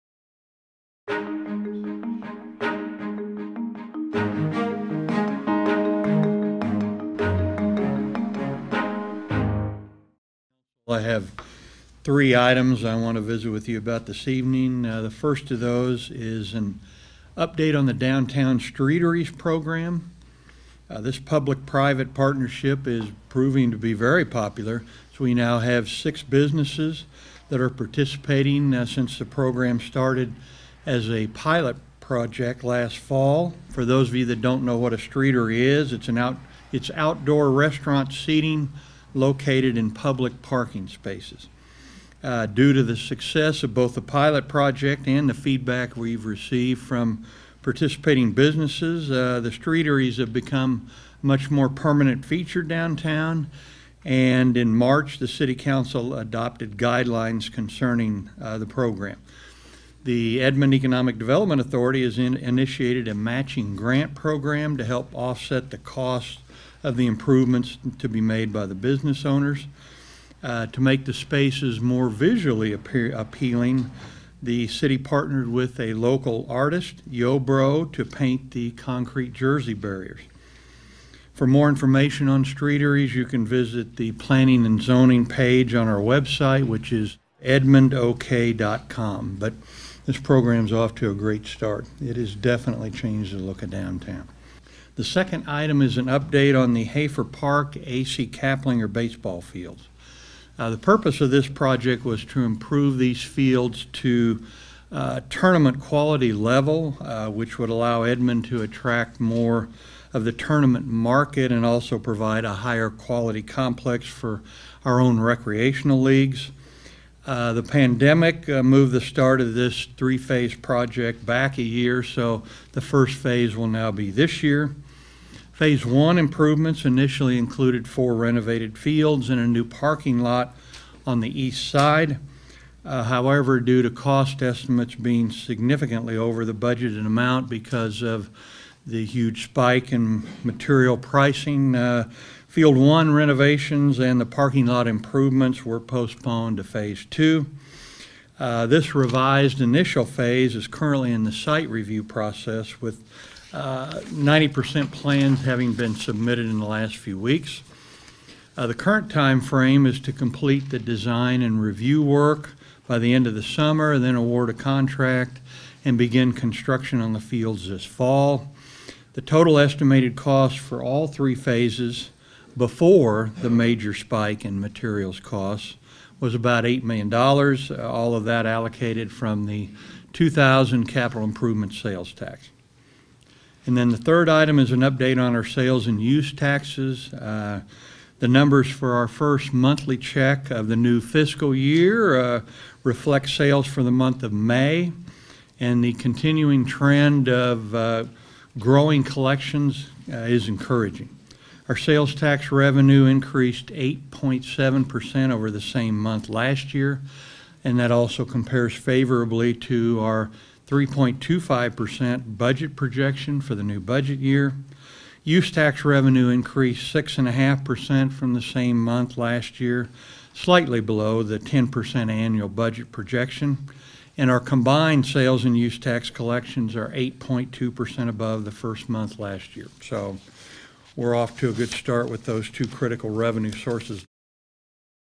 City Manager's Progress Report: July 2021
Items Discussed:
- Downtown "Streateries" Program (00:14)
- Hafer Park / AC Caplinger Baseball Fields (01:34)
- Sales / Use Tax (03:03)

- Downtown "Streateries" Program
This public-private partnership is proving to be very popular, as six downtown businesses are now participating since the program started as a pilot project last fall.  A “Streatery” is outdoor restaurant seating located in public parking spaces.  Due to the success of the pilot project and feedback received from participating businesses, the streateries have become a more permanent feature and guidelines were adopted by the City Council in March.  The Edmond Economic Development Authority (EEDA) has initiated a matching grant program to help offset the costs of the improvements made by the business owners.  To make the spaces more visually appealing the City partnered with a local artist, Yo Bro, to paint the concrete jersey barriers.

For more information on streateries you can visit the Planning and Zoning page on our

- Hafer Park / AC Caplinger Baseball Fields
This project will improve these fields to “tournament quality”, allowing Edmond to attract more of the tournament market and provide a higher quality complex for recreational leagues.  The pandemic impact moved the start of this three-phase project back a year to this year.

Phase I improvements initially included 4 renovated fields and new parking on the east side.  Due to cost estimates being significantly over the budgeted amount because of material pricing, Field #1 renovations and the parking lot improvements were postponed to Phase 2.  This revised initial phase is currently in the site review process, with 90% plans submitted in early July.

The current time frame is to complete the design/review work by the end of the summer, then award a contract and begin construction this fall.  The total estimated cost for all three phases, before the major spike in material costs, was $8M, allocated from the 2000 capital improvements sales tax.

- Sales / Use Tax
The numbers for our first monthly check of the new fiscal year reflect sales for the month of May.  The continuing trend of growing collections is encouraging.  Sales tax revenue increased 8.7% over the same month last year, comparing to a 3.25% budget projection for the new budget year.  Use tax revenue increased 6.5% from the same month last year, slightly below the 10% annual budget projection.  Combined collections are 8.2% above the first month last year.